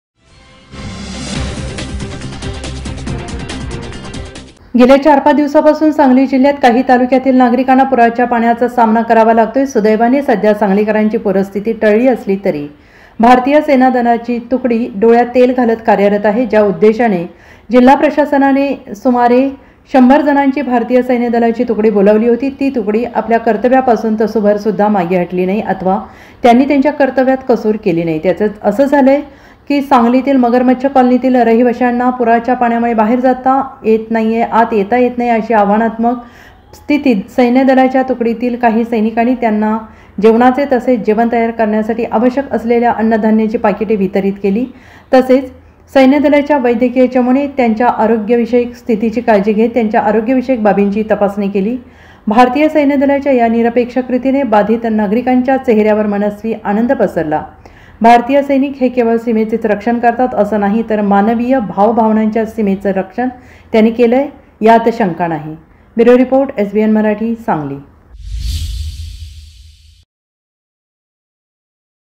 SANGLI | सांगली जिल्ह्यातील पुरावर भारतीय सेनादलाचे डोळ्यात तेल घालून लक्ष
सांगली जिल्ह्यातील पुरावर भारतीय सेनादलाचे डोळ्यात तेल घालून लक्ष, आपल्या कर्तव्यापासून तसभूरही मागे हटले नाही.